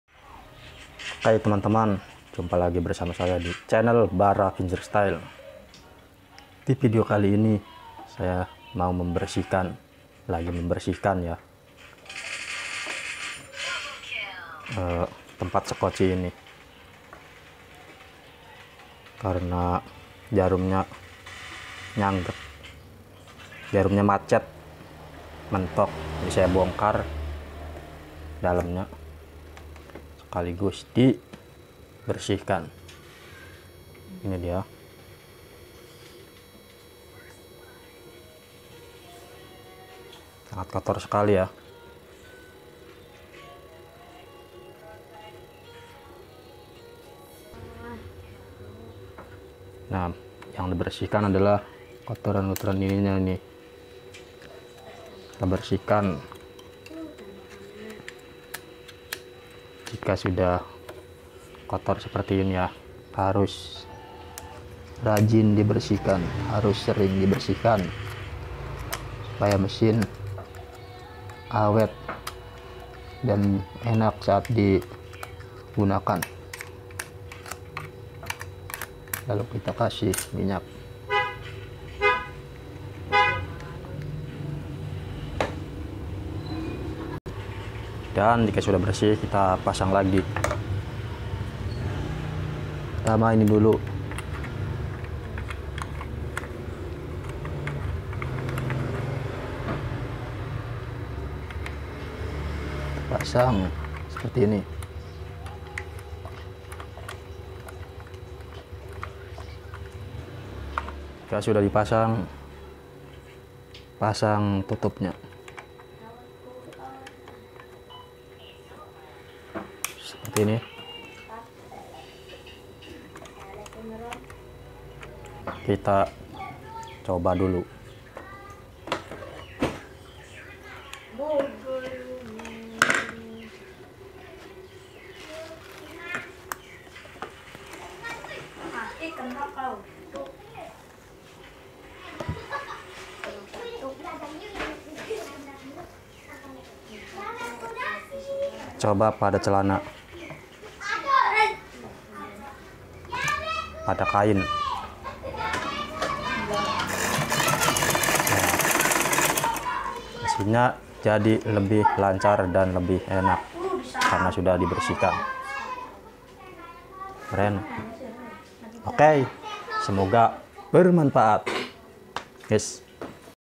cara memasang bulan² mesin jahit butterfly
berikut adlh video cara membersihkan dan memasang bulan² mesin jahit butterfly semoga bermanfaat